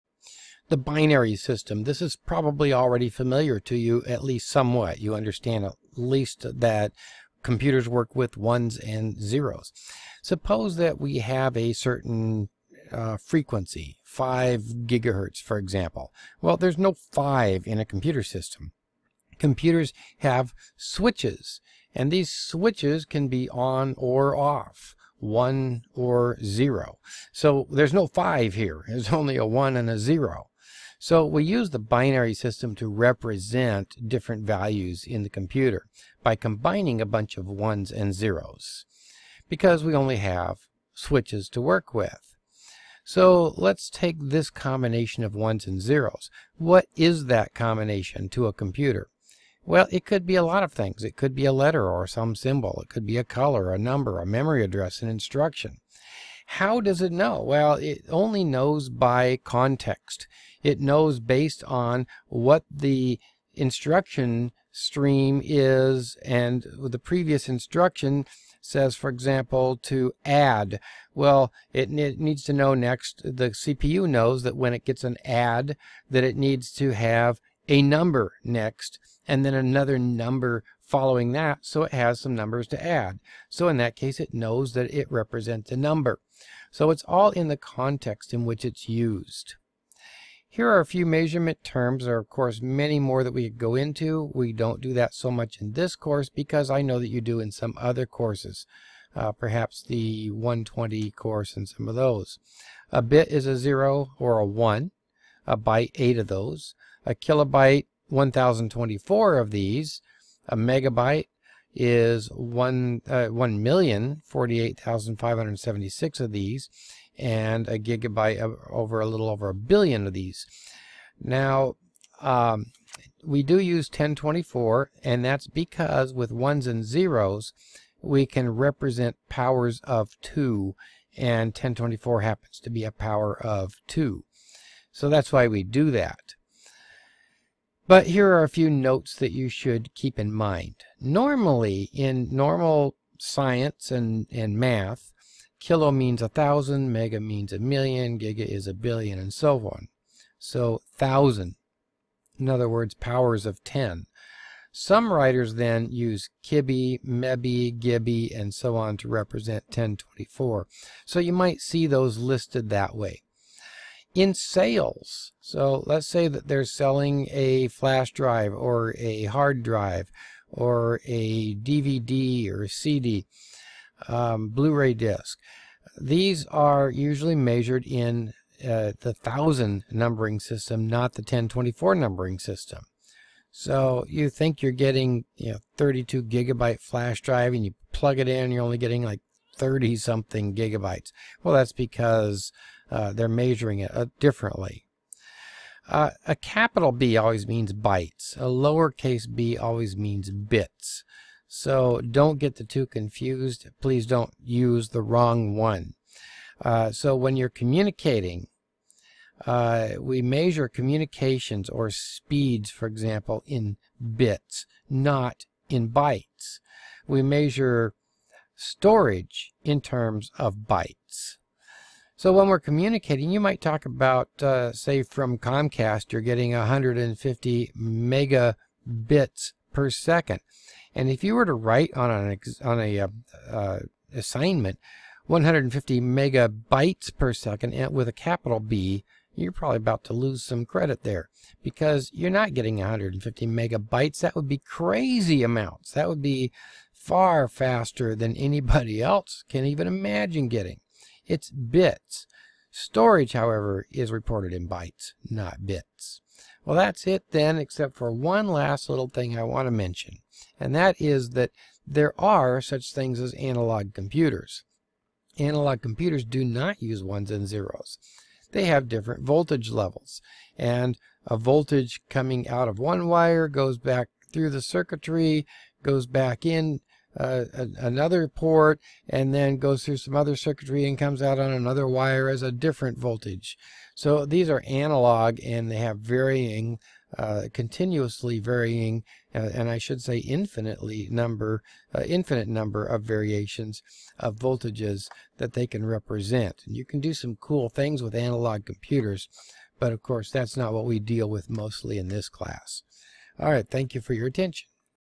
2-06 Binary usage
This video is part of an Intro to Networking class, which is a brief survey of many networking terms and concepts. It is not meant as an in-depth tutorial on this topic.
If you find inaccuracies, please let me know. I want to be sure they are as accurate as I can make them.